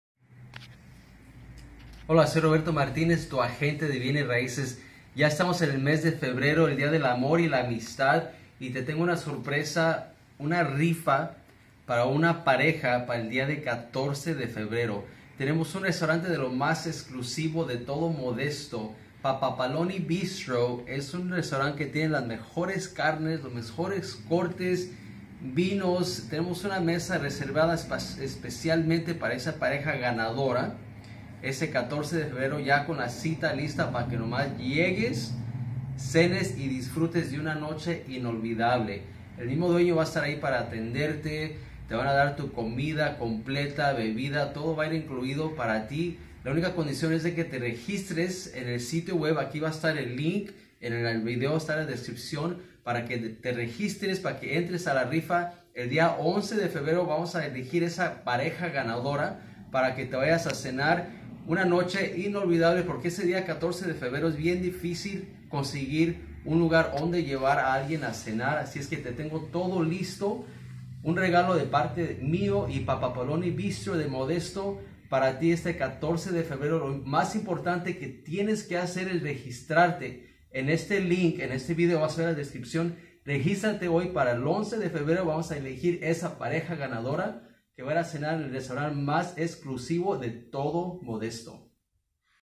Rifa para el 14 de Febrero
Participa Registrandote en este link

Esta cena es  para 2 personas  el 14 de Febrero, el consumo es solamente en el Restaurante Papappoloni Mediterrian Bistro 2501 McHenry Ave. Modesto (no para llevar) incluye cualquier platillo, incluye una botella de vino tinto ( restricciones aplican ). La cena será sin costo alguno para los ganadores y no transferible. El sorteo será el día Viernes 11 de Febrero del 2022 a las  2:00 pm. por FB Live en el canal de Roberto Martínez.